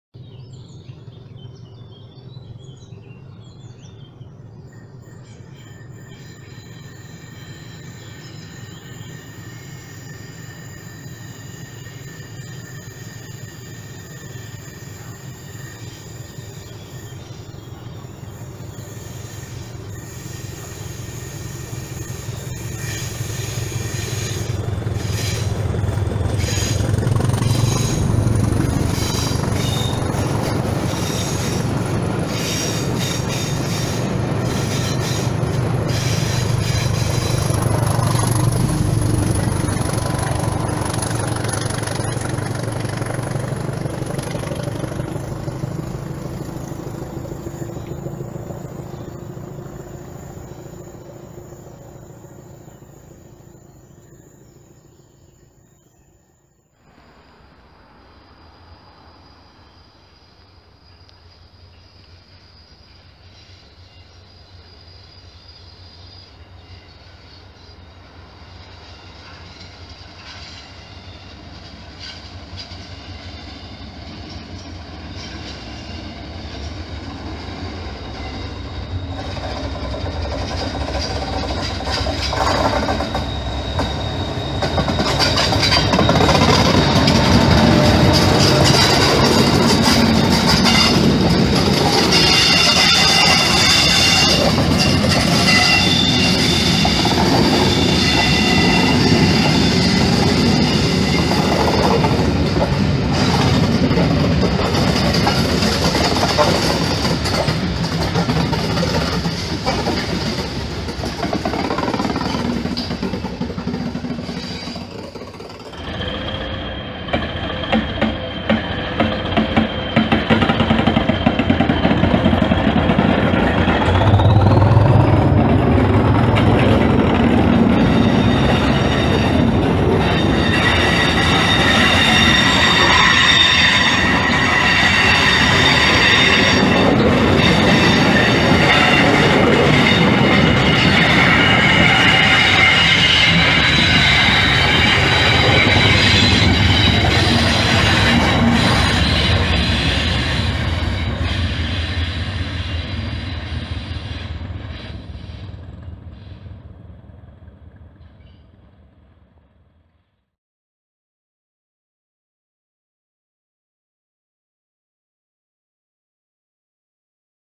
(HD) 1001 'Hastings Thumper' thrashes on the Oxford Rover Railtour - 27/05/17
1001 'Hastings Thumper' is seen passing Filsham Valley Footbridge, & entering and exiting West St Leonards with the 'Oxford Rover 'Railtour on 27/05/17